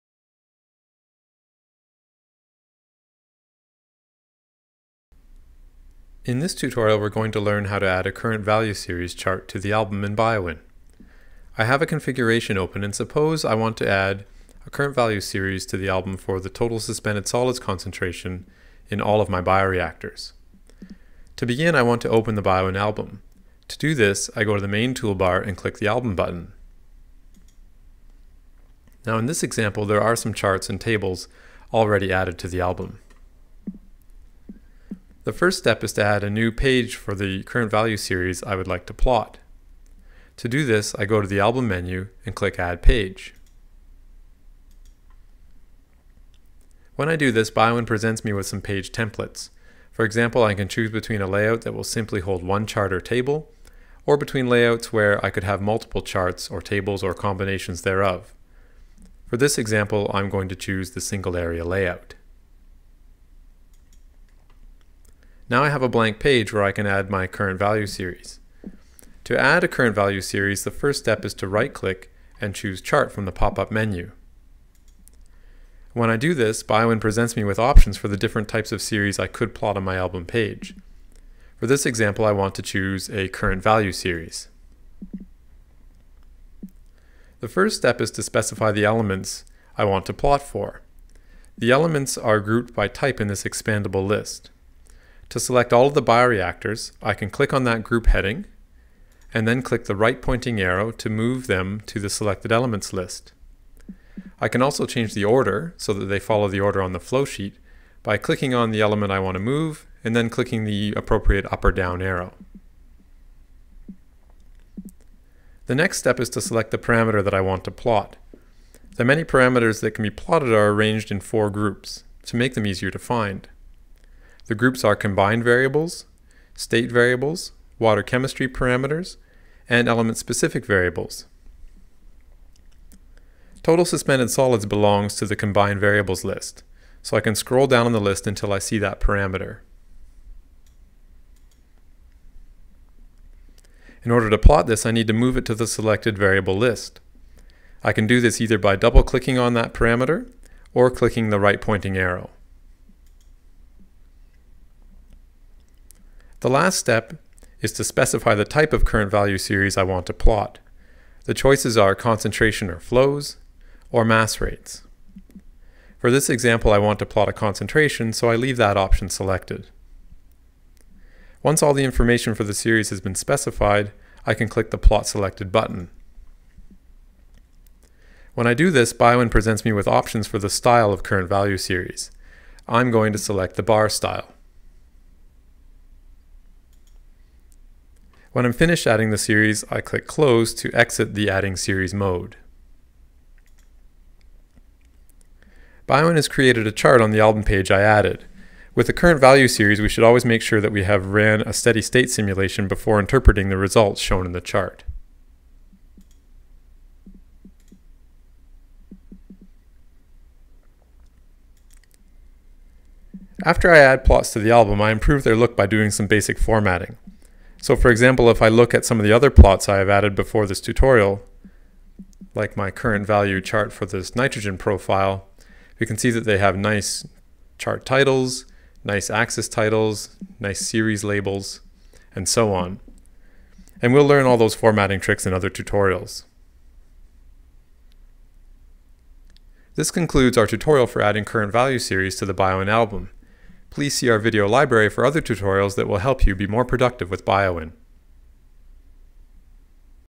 Adding a Current Value Series with BioWin
A tutorial on adding a current value series to the BioWin Album.